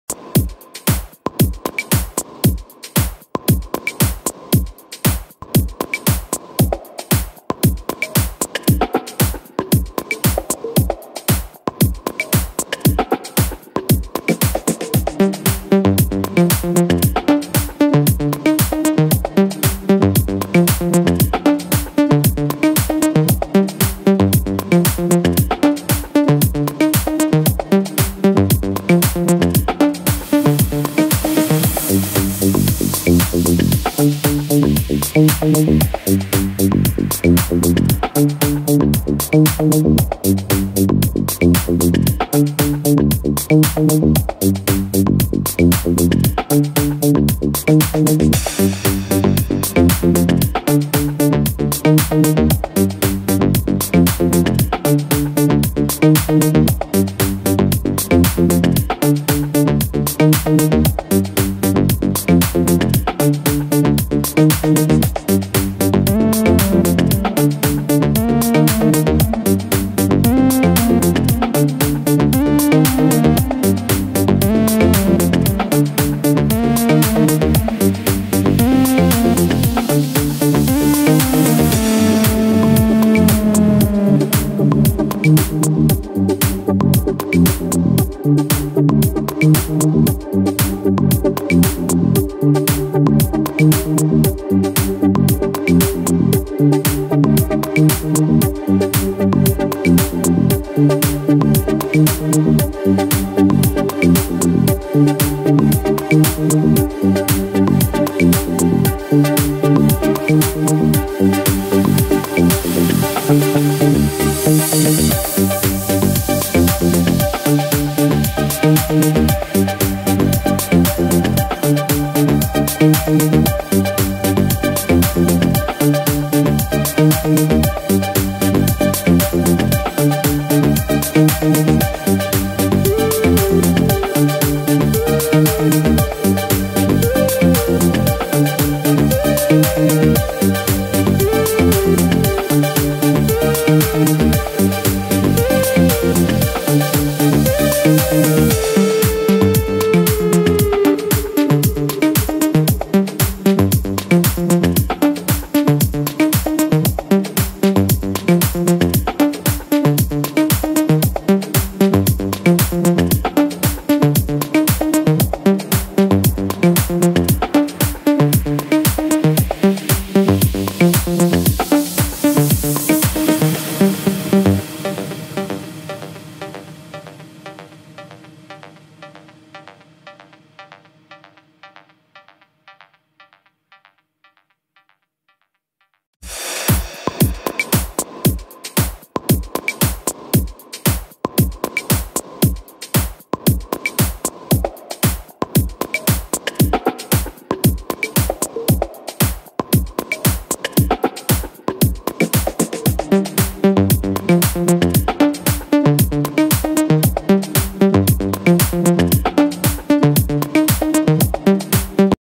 Kali Linux Tools : SSLScan - KuKuAirs
SSLScan in Kali Linux.

SSLScan is a tool to check the SSL service.

My computer Operating System is Windows 8.1

Easy to install for a beginner lesson

※Click the gear and videos, HD Watch in high quality.

Created by KuKuAirs.